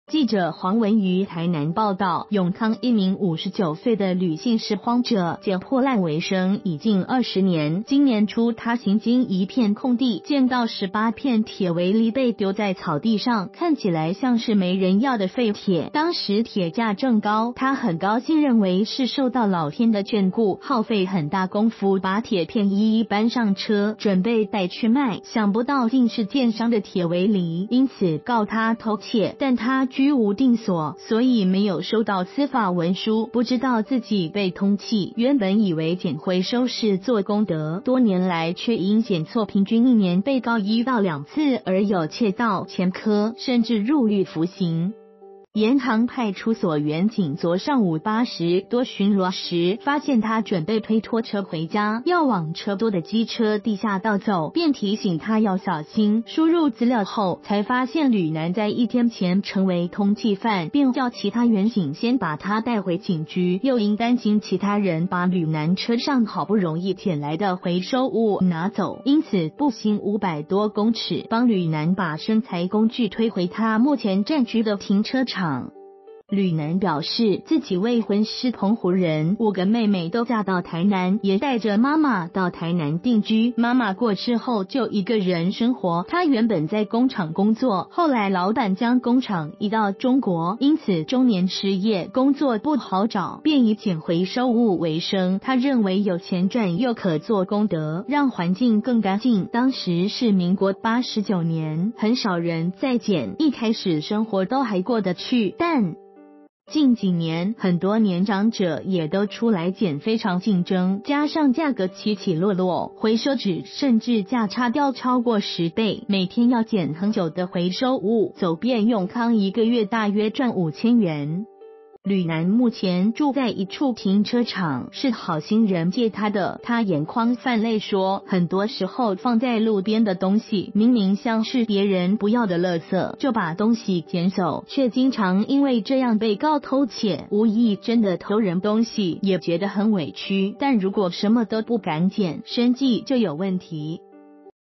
拾荒20年月薪5k 他「撿錯」鐵片成通緝犯
〔記者黃文瑜／台南報導〕永康一名59歲的呂姓拾荒者，撿破爛為生已近20年，今年初他行經一片空地，見到18片鐵圍籬被丟在草地上，看起來像是沒人要的廢鐵，當時鐵價正高，他很高興認為是受到老天的眷顧，耗費很大工夫把鐵片一一搬上車，準備帶去賣，想不到竟是建商的鐵圍籬，因此告他偷竊，但他居無定所，所以沒有收到司法文書，不知道自己被通緝，原本以為撿回收是「做功德」，多年來卻因「撿錯」平均一年被告1到2次而有竊盜前科，甚至入獄服刑。